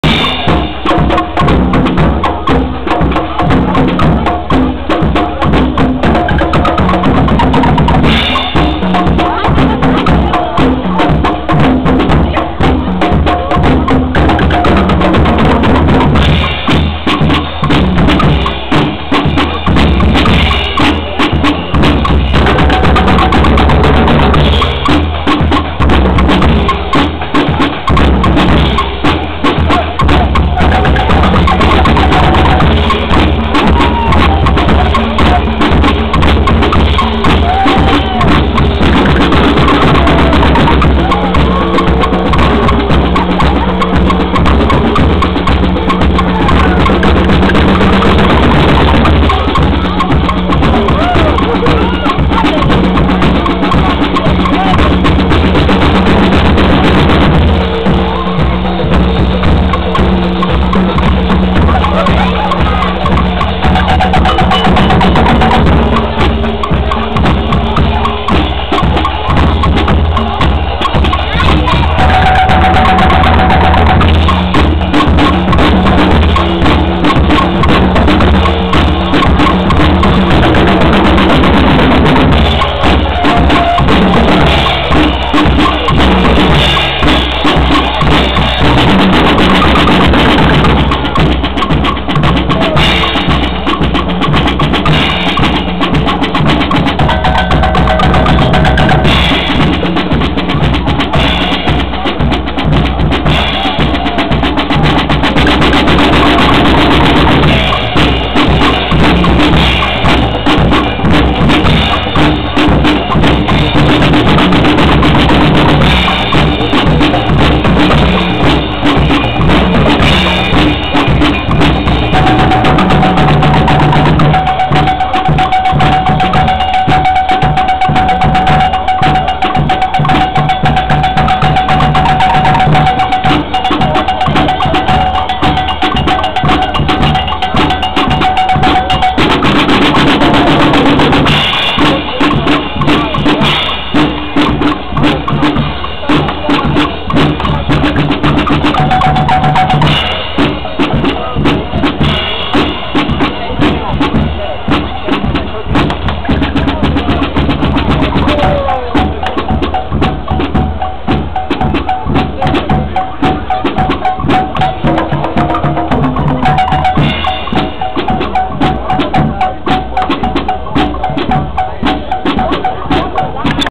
Milkauer Schalmeien auf Schützenfest Hohenfichte
Hohenfichte Schützenfest am 27.07.08
Die Milkauer Schalmeien warten vor dem Riesenrad, wo die Schützenkönige und deren Königinnen mehrere Runden fahren.